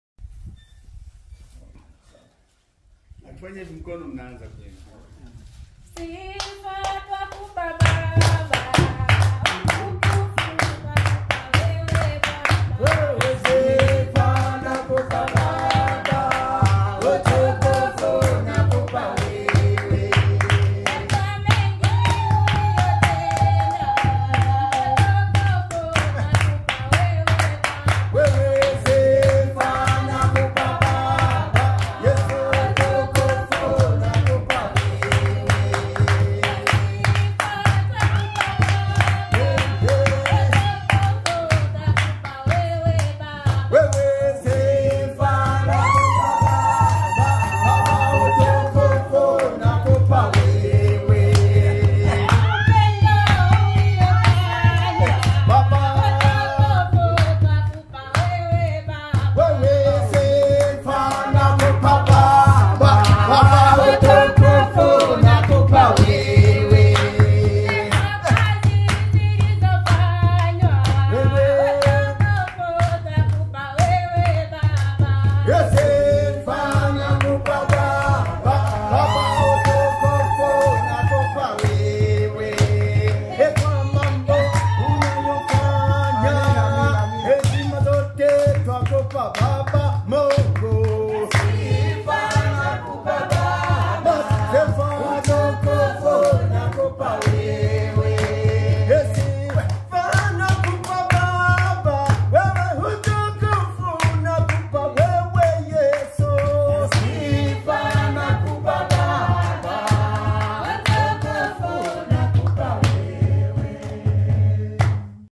Glorious spontaneous singing at Kyanshenge Church, Lweru Tanzania
When ARDFA conducted a project visit to inspect the church roof our donors funded in Kyanshenge, an offer was made to some of the representative beneficiaries, whether they would like to offer a song to friends in Australia. The result was an outstanding impromptu singing that will truly lift your spirit. Church buildings in Tanzania are used for multipurpose and support the whole village by offering a safe and functional all-weather meeting place. ARDFA Aug 2022